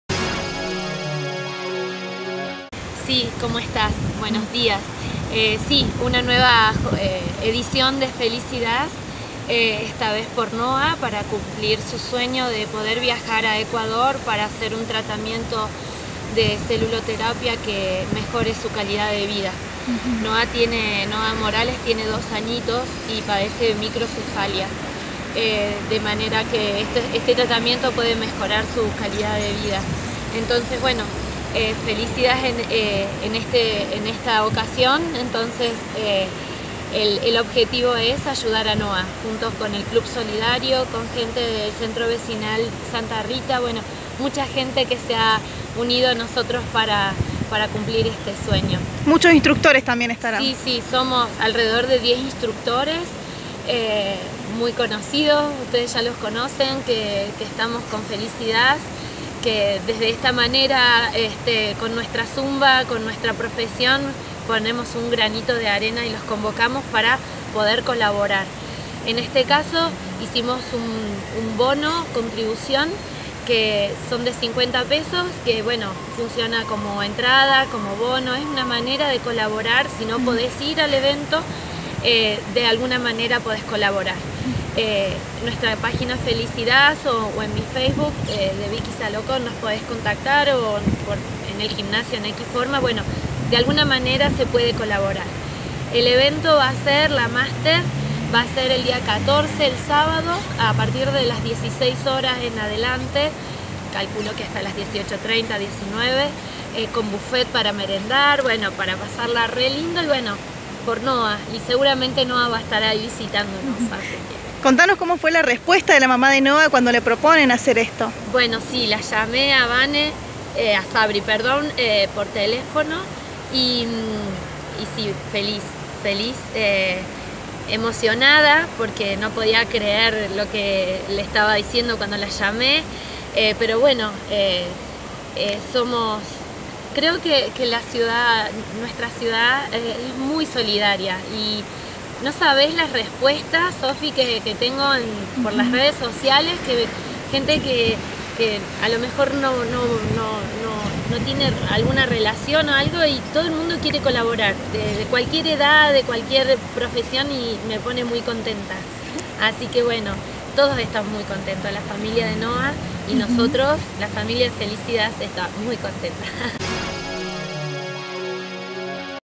ZUMBA SOLIDARIA POR NOAH
La instructora de zumba Vicky Zalloco brindará una clase a beneficio de Noah Morales, un bebé que nació con microcefalia. Su mamá Sabrina Castro encontró un tratamiento en Ecuador para mejorar su calidad de vida.
Enterate cómo colaborar en esta nota